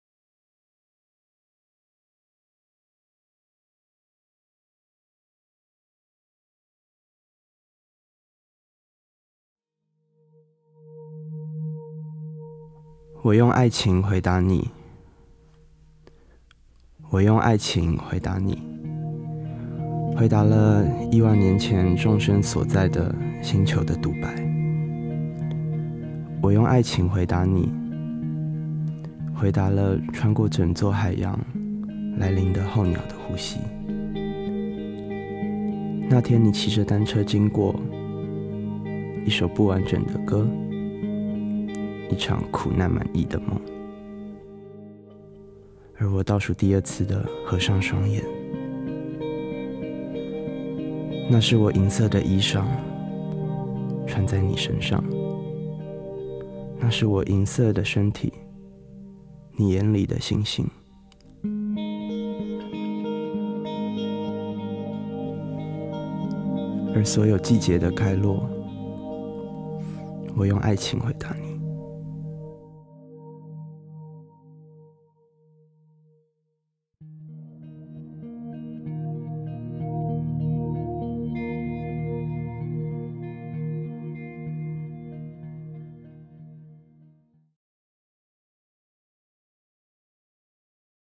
【2019詩人節特別企劃】為你讀詩[第一輯1]─許含光
文字．朗讀｜許含光
攝影．剪輯｜林煜幃
錄音師 Recording Engineer｜陳以霖 Yi Lin Chen
錄音室 Recording Studio｜大小眼錄音室 Twin Eyes Studio

享受一首詩的方式，不只是用雙眼閱讀，還可以張開耳朵聆聽。
每一種斷句和留白，都是情感的呼吸。

今年博客來詩人節特別邀請音樂創作者、作詞家與詩人，
以自己感到最舒服的方式，獨家為你讀詩。

更透過紙上專訪，
分享他們欣賞的詩人、最愛的詩集，
以及創作遇到瓶頸時，面對的方式。

12支獨家讀詩影片，
希望你會喜歡。